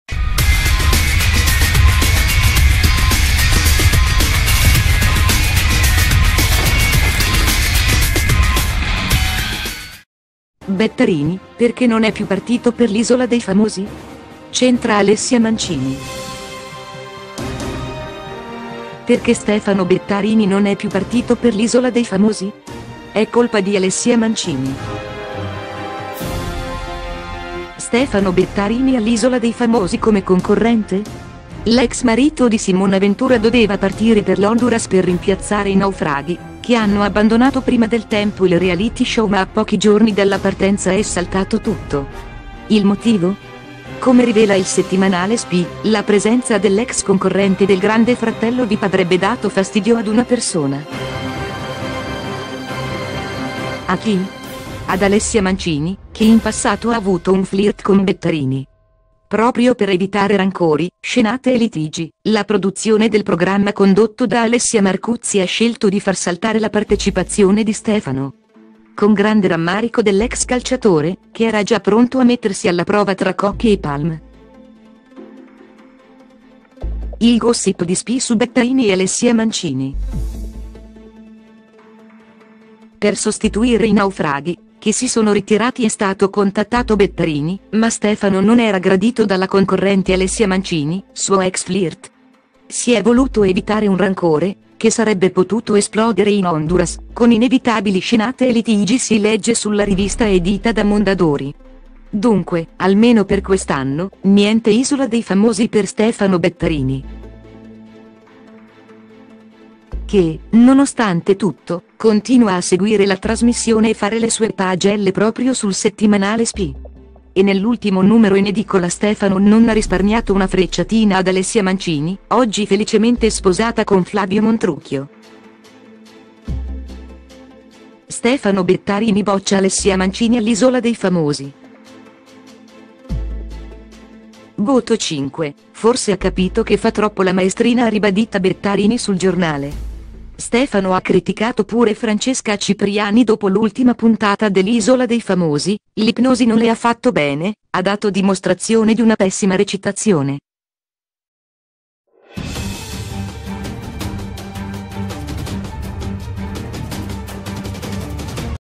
Bettarini, perché non è più partito per l’Isola dei Famosi? C’entra Alessia Mancini
Perché Stefano Bettarini non è più partito per l’Isola dei Famosi? È colpa di Alessia Mancini